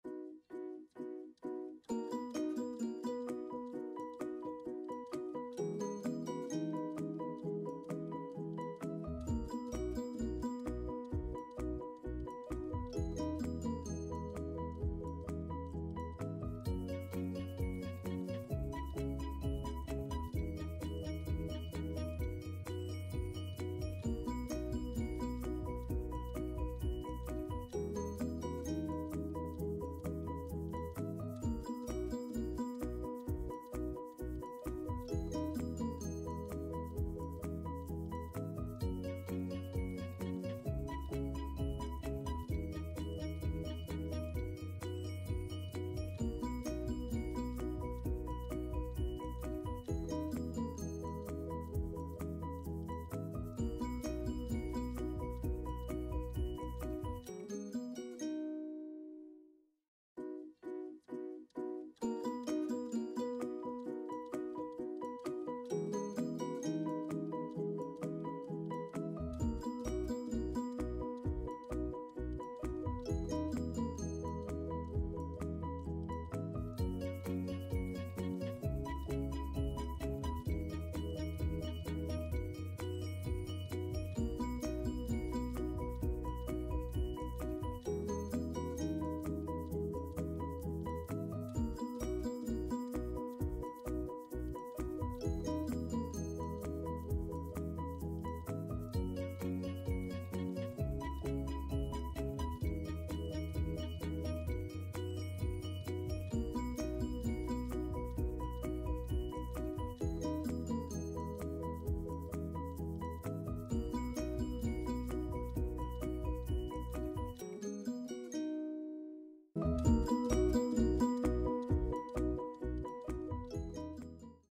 How to paint cherries using a bottle cap / Easy bottle cap painting for begginers / Easy painting.
How to paint cherries using bottle cap. Easy painting technique to paint cherries.